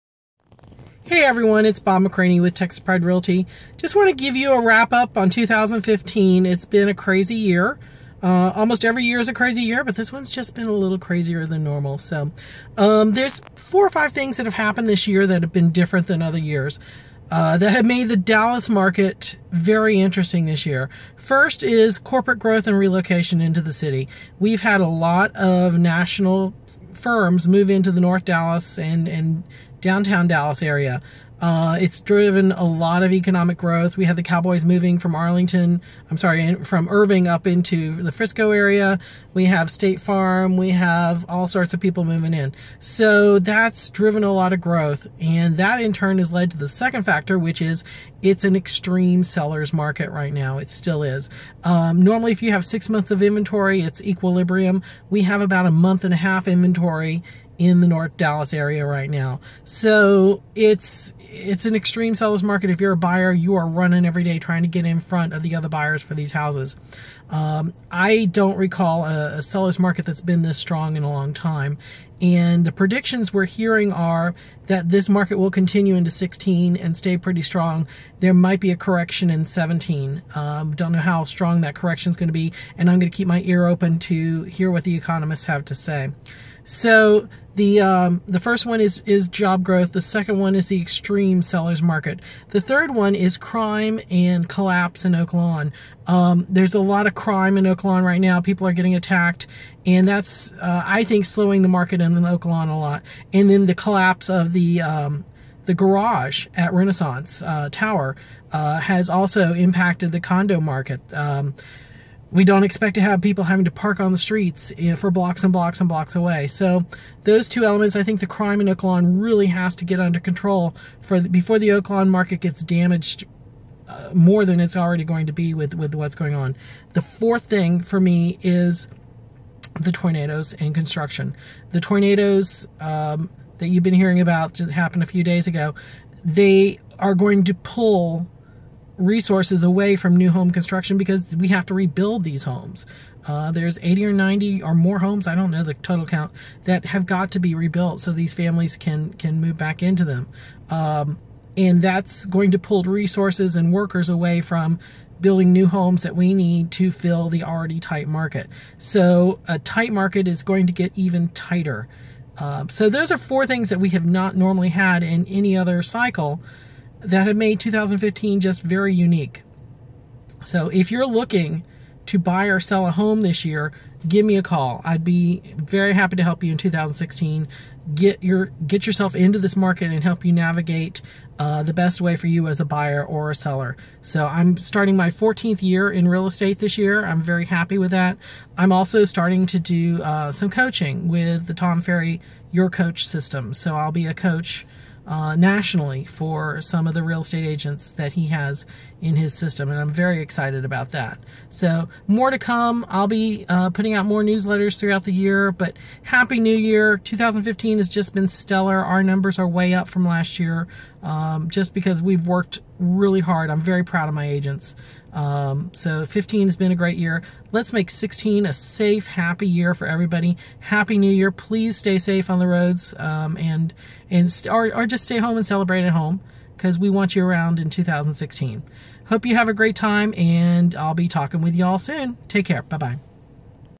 End of Year Real Estate Review for Dallas!
It's been an unusual year in real estate.  Several factors have made 2015 a unique year!   If you're looking to buy or sell in 2016 please let me help you.